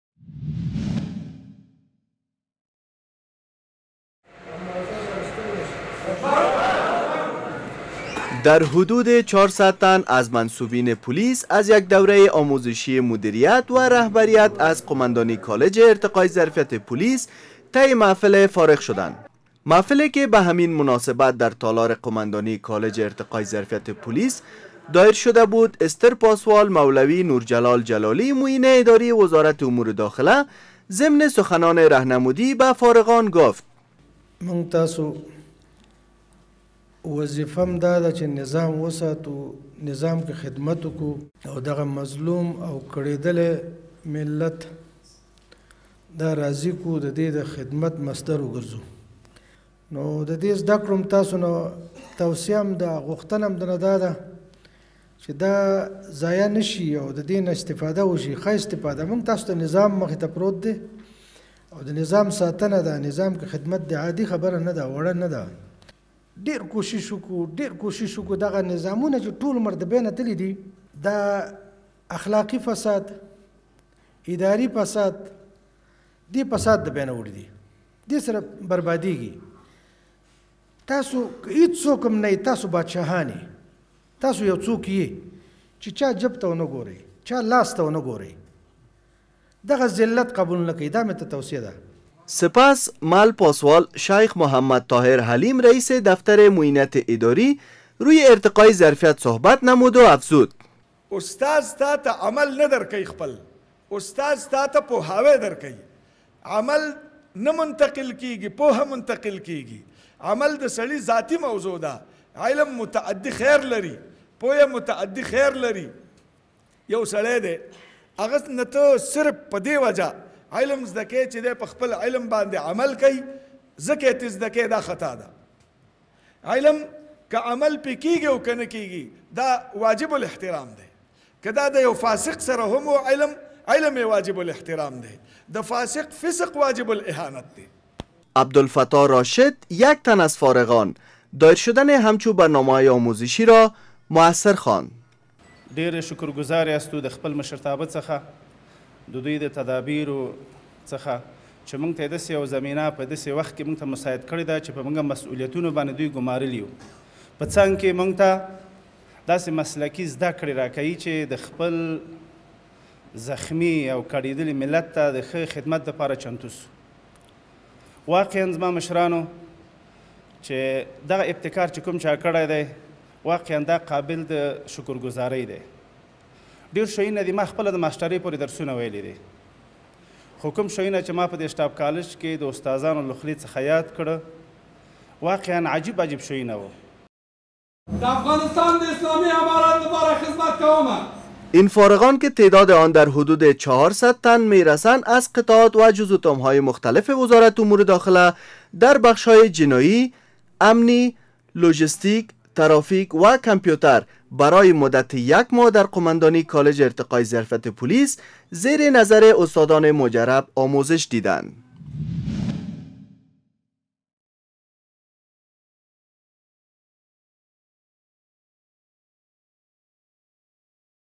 فراغت نزدیک به ۴۰۰ تن از قوماندانی کالج ارتقای ظرفیت پولیس
برنامه های وزارت امور داخله را از آدرس های متذکره میتوانید ببینید

Ministry of interior Afghanistan